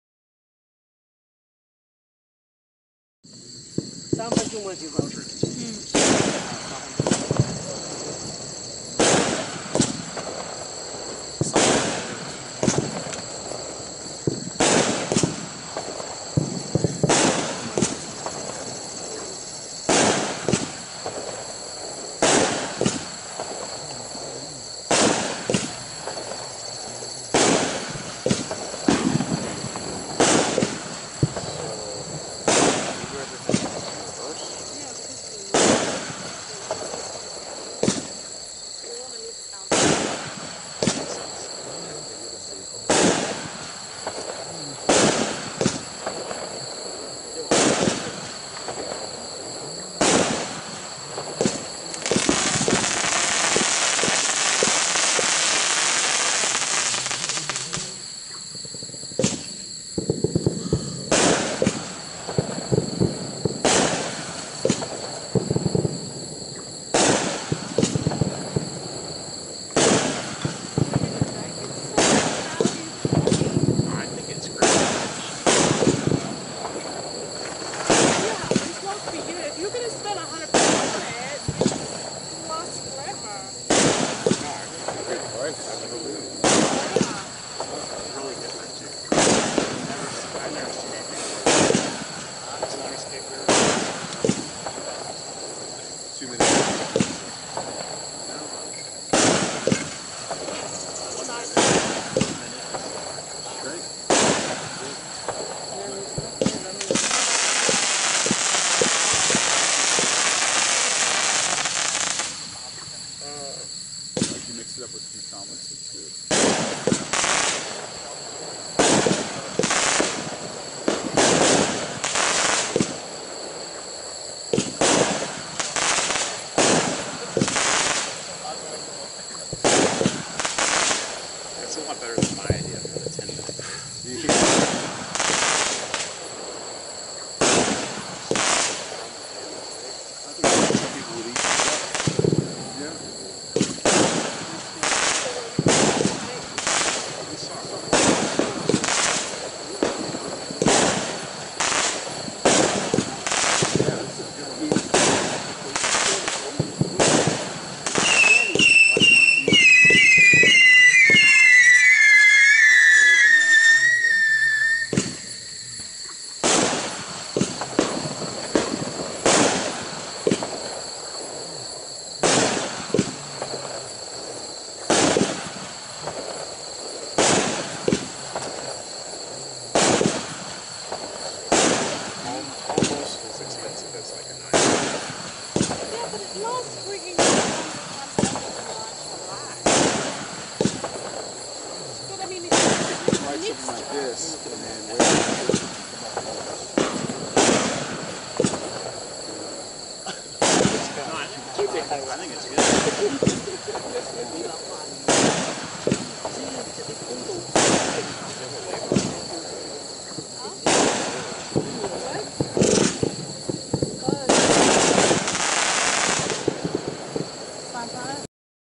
3 Minute Mayhem | 100-Shot by Shogun
Break all the records with this 3-minute long, action-packed show-in-a-box featuring fanned and straight effects with amazing color combinations! 100-shot, 500-gram cake from Shogun, Available at the Iowa Fireworks Farm.